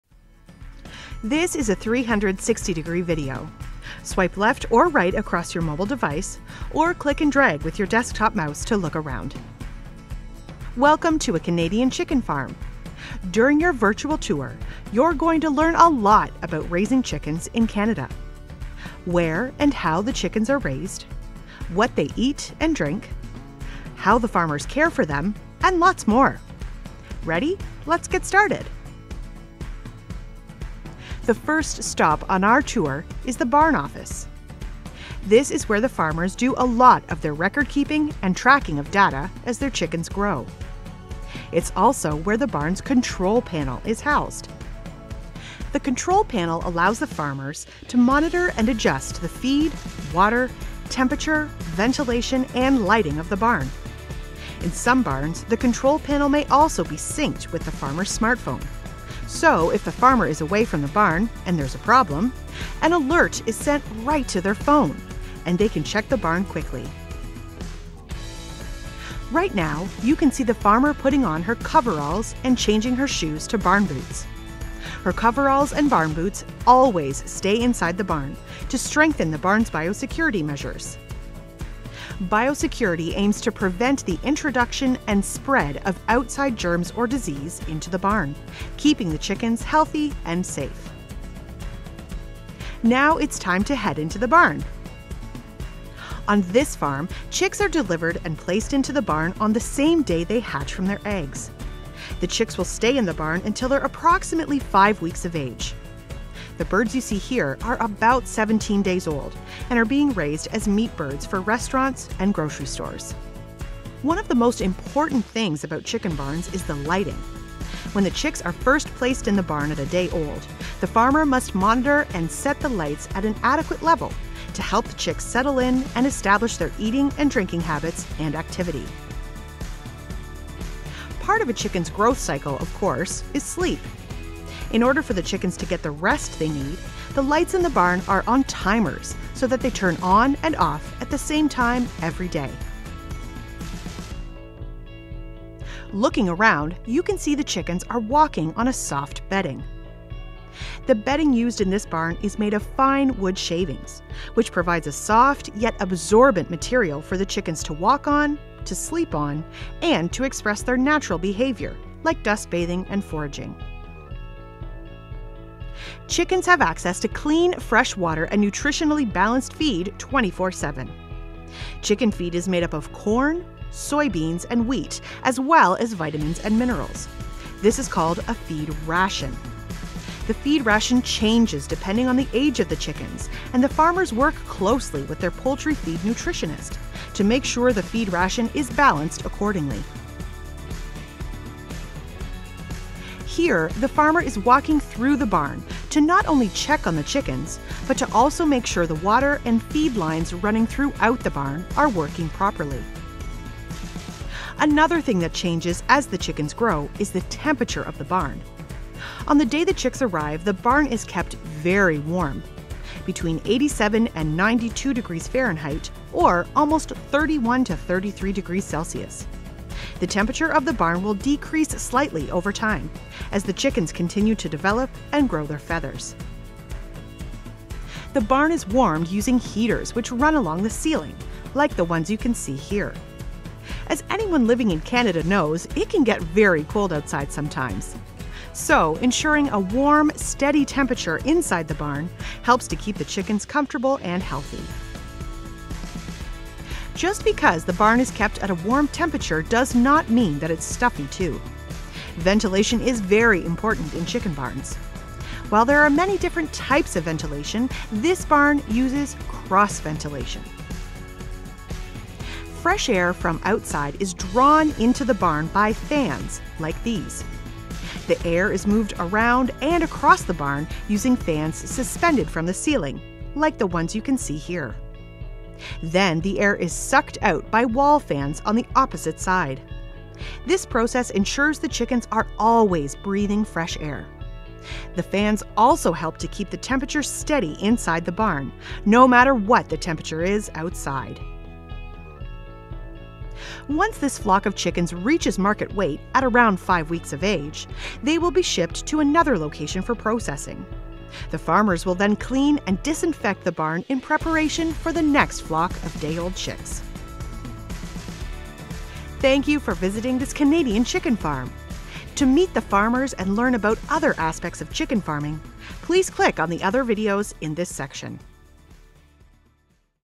FarmFood360° Virtual Reality Chicken Farm Tour
In this 360° video tour, you’ll get to see inside a working Ontario broiler chicken farm. These are chickens that are raised for meat. You’ll learn how chickens are cared for from the time they arrive as newly hatched chicks until they are ready for market. You’ll also learn what they eat, where they live, and how chicken farmers care for them. to tour other types of Canadian farms and food processing facilities.